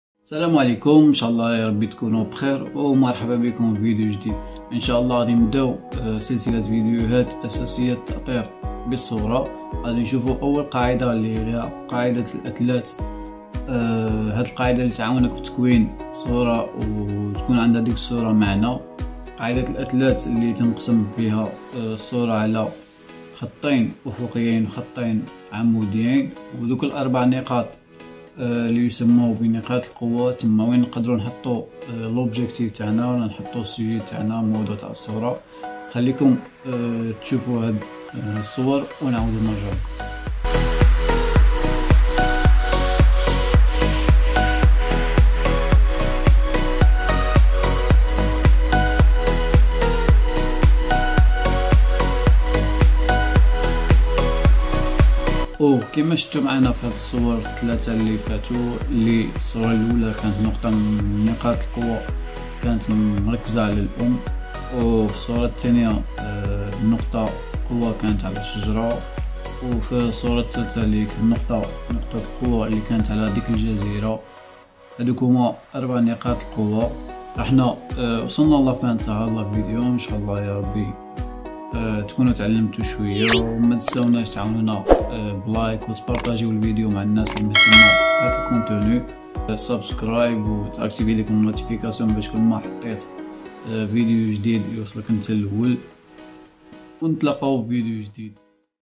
أساسيات التأطير : قاعدة الأثلاث / Les bases du cadrage : la règle des tiers
في هاذ السلسلة راح نتعلمو كيفاه نديرو إطار شباب يجذب الإنتباه للموضوع تاعنا